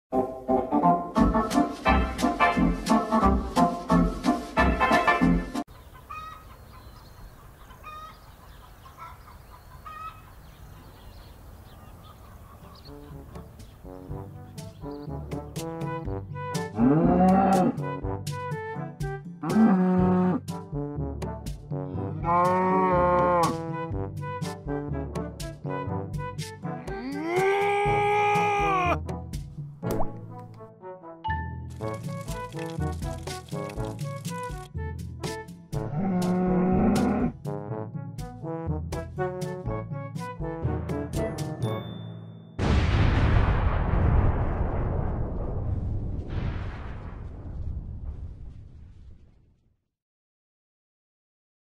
Where Does Milk Come From?
This is a cartoon about milk or something... YES.